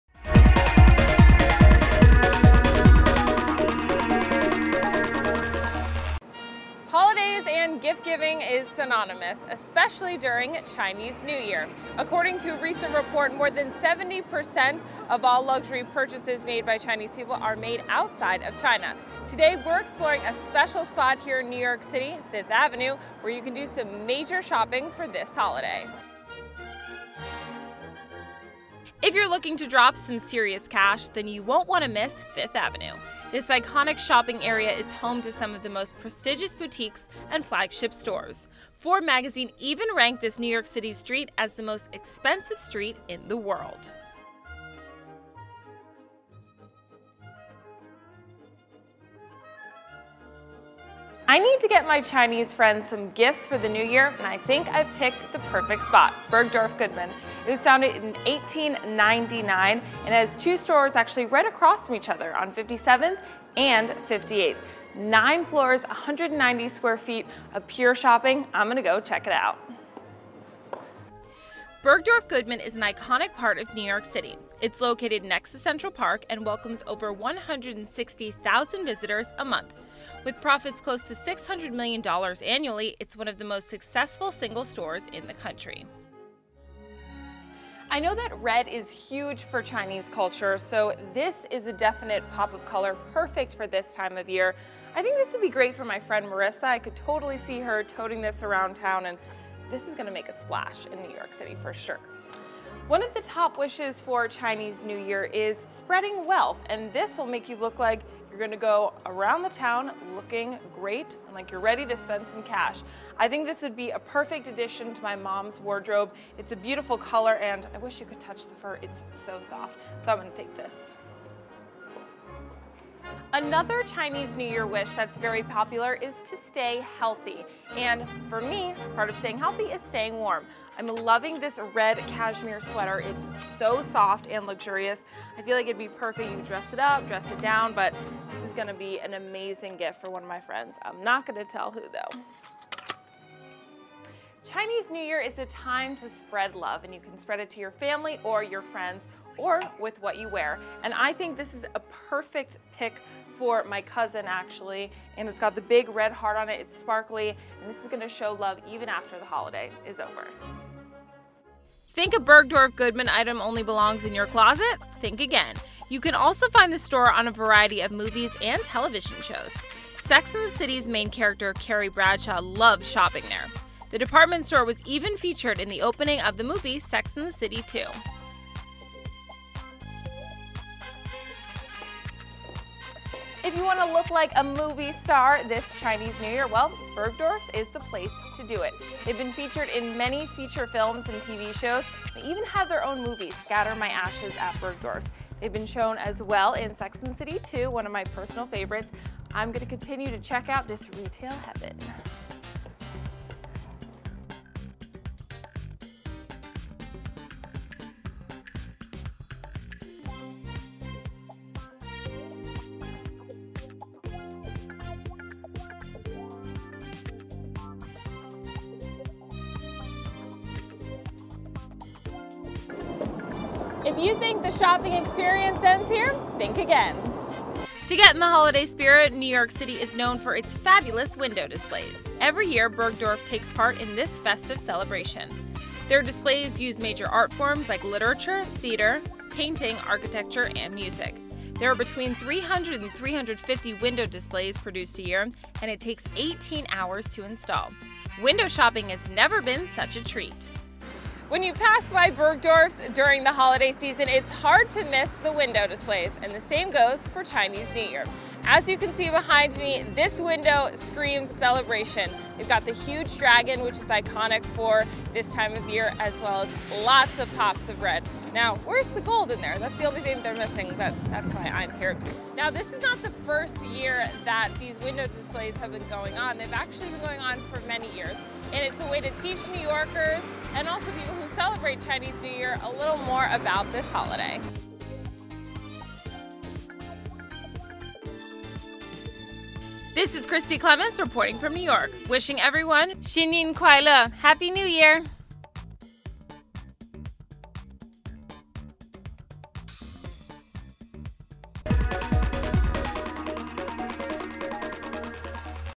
Window Shopping at Bergdorf Goodman
Host Christie Clements takes you on a tour of Fifth Avenue's Bergdorf Goodman. Team up with her as she picks out Chinese New Year gifts for family and friends.